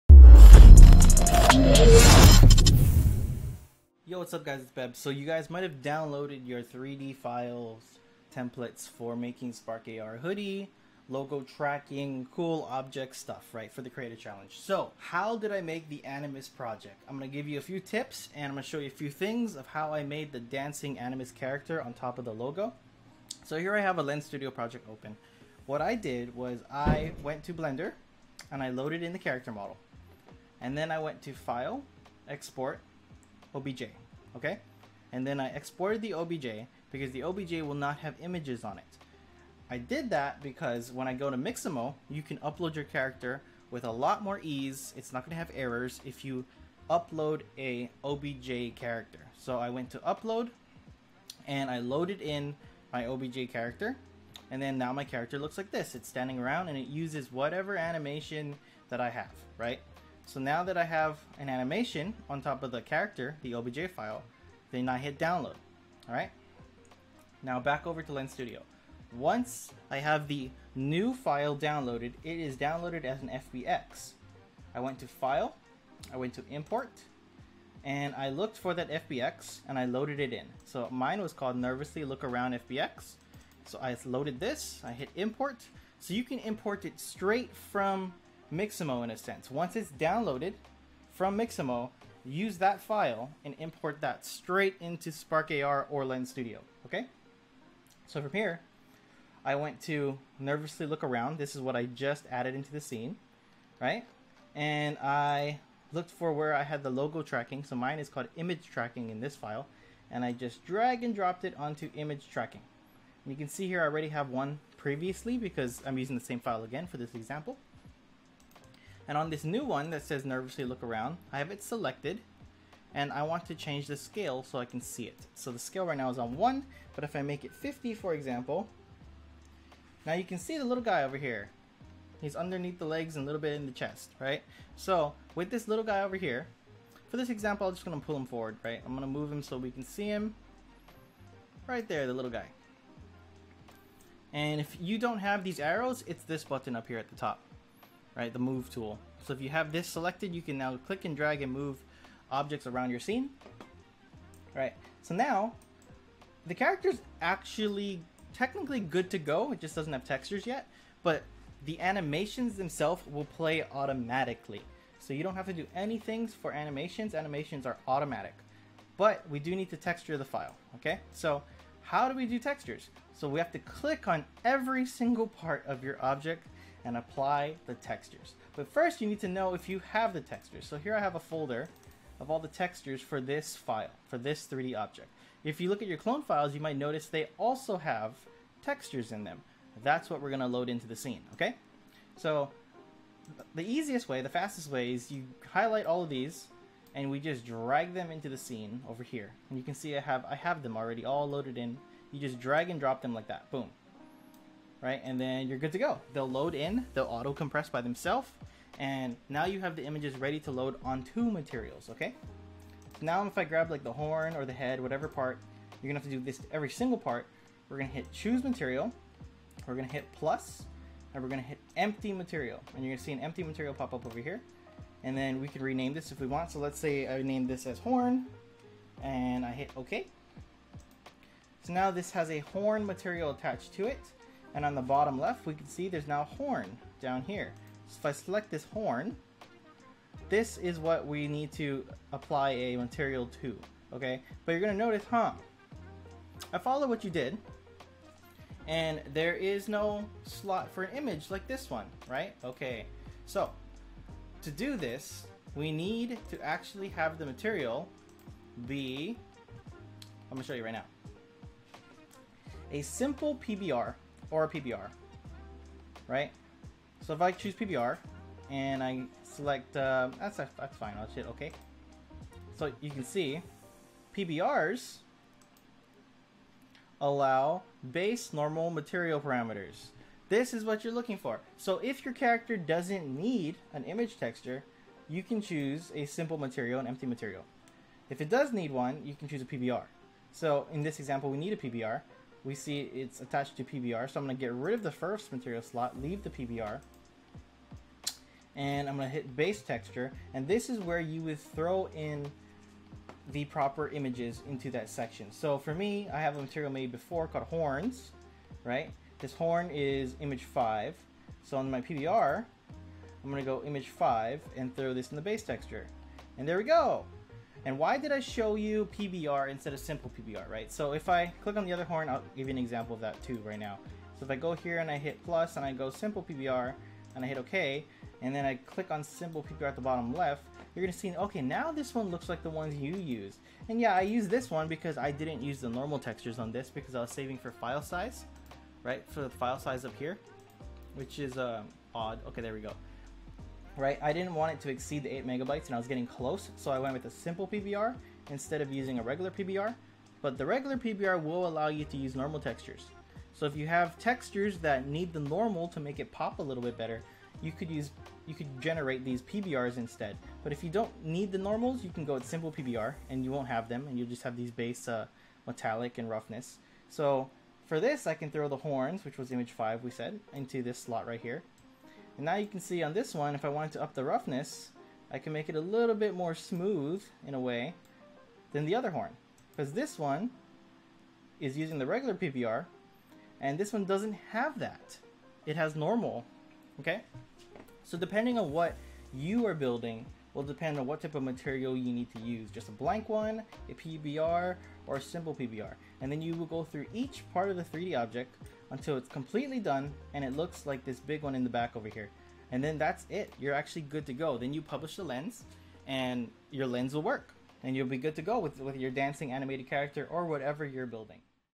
Working with Animations and Materials in Spark AR and Lens Studio
Hey fam, Beb here. This video explains how to work with materials and animations. In this example I explained the process of how I made a dancing animus character.